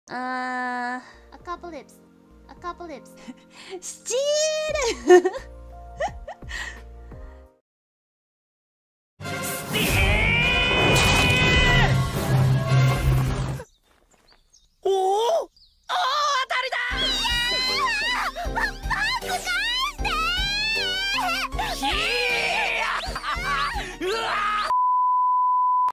【Hoshifuri Iku】STEALLL 【PRISM Project】
Here's another KonoSuba line from Iku

Hoshifuri Iku is a VTuber from PRISM Project Gen 1. Don't forget to check out her channel!

Fellow PRISM Gen 1 members

Recently-debuted PRISM Gen 2 members